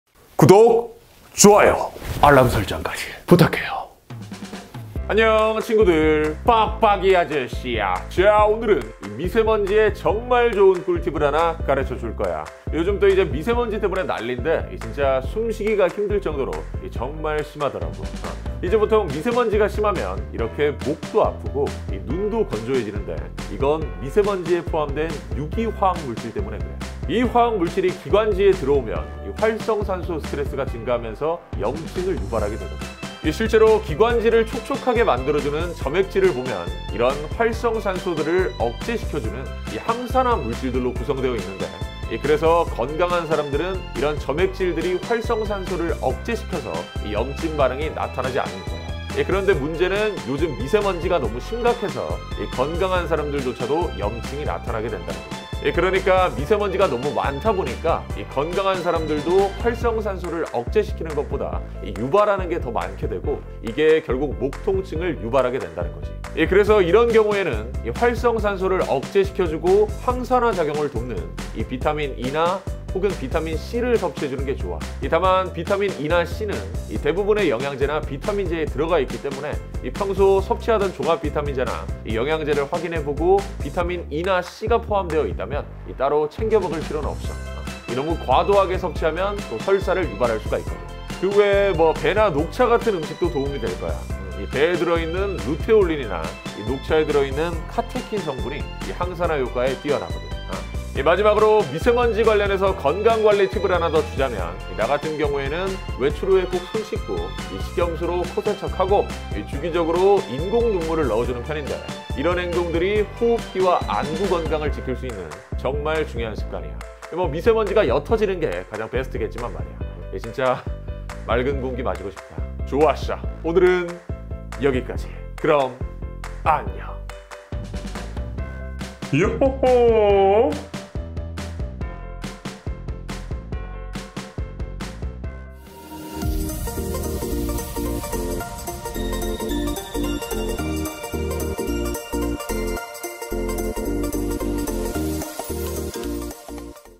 반드시 알아야 할 미세먼지 대처법 (현직 약사의 조언)
미세먼지가 심할 때 기침이 나오거나 숨쉬기 어렵다면 꼭 참고해 주세요